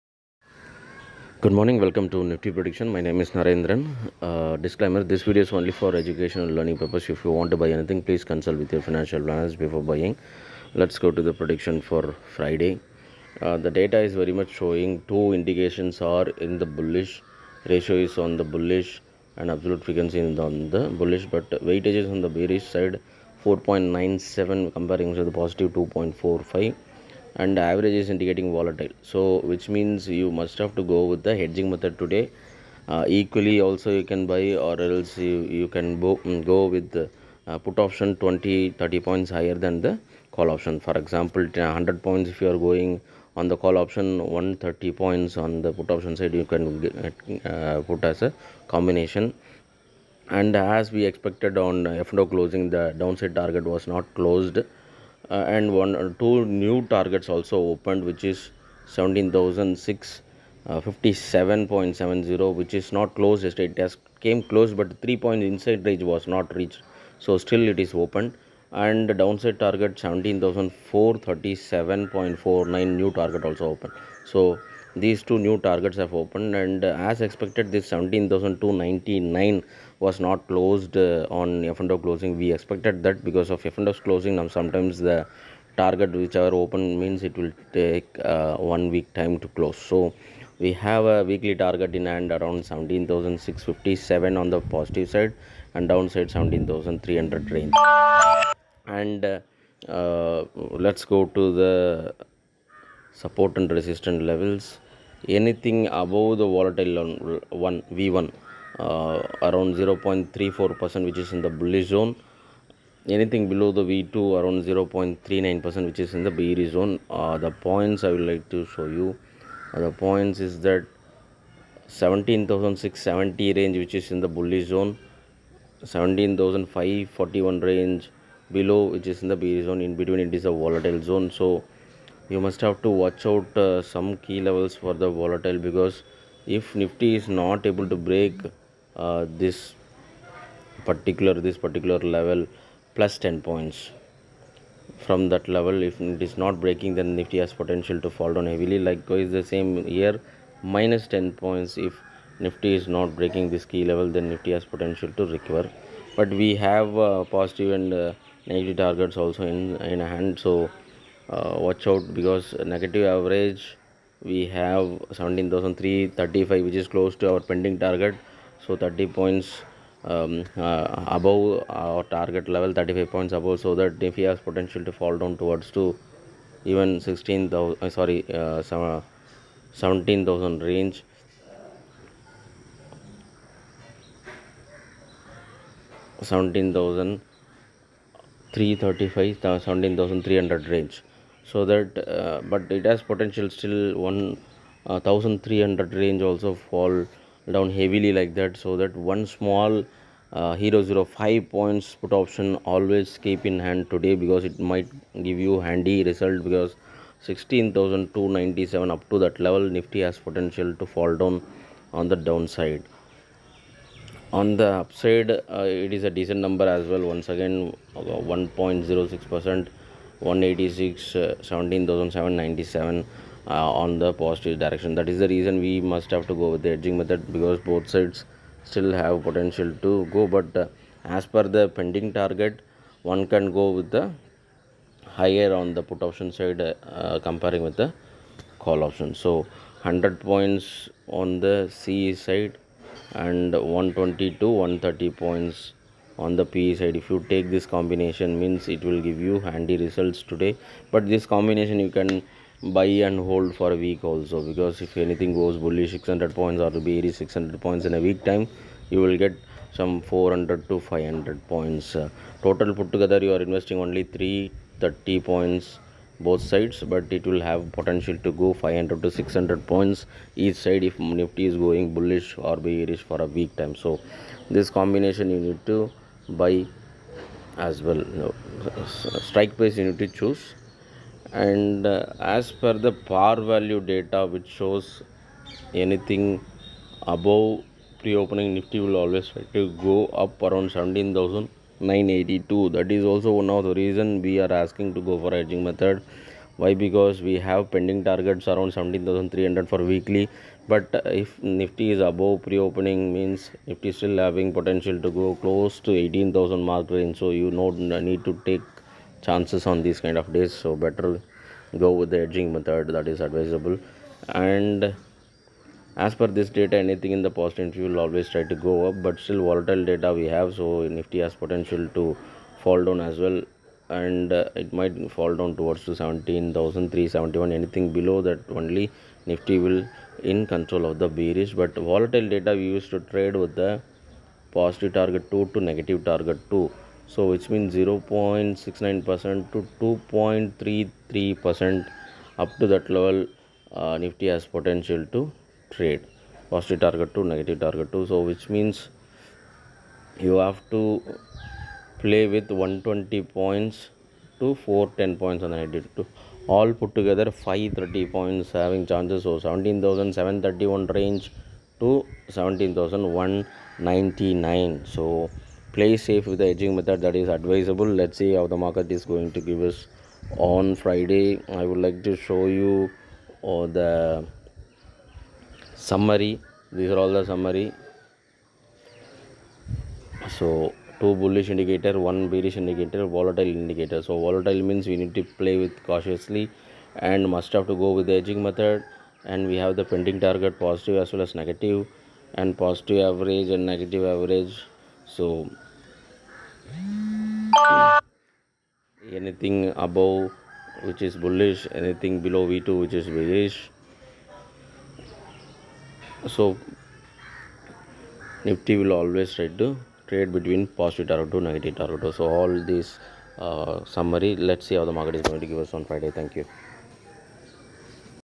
NIFTY PREDICTION ANALYSIS FOR FRIDAY 03 FEB 2023
Todays data we have 2 bullish indicators 1 bearish indicator 1 volatile indicator

So that must keep hedging method

Note: we have both sides pending targets in hand for weekly  positive side 17,657.70 negative side 17,437.49  & 17,299.19

POSITIVE AVERAGE 17,797.07

Anything above V1 17,670.28 is in the bullish zone

Anything below V2 17,541.72 is in the bearish zone

Anything in between those two levels are the volatile zone

NEGATIVE AVERAGE 17,335.68

Small and safe traders must go with hedging method equally

Pre opening anything above PAR VALUE nifty will try to go bullish at the same time we have 2 bearish targets for the week in hand so only must have to go with heading method

Due to volatile PT2 to NT2 is still possible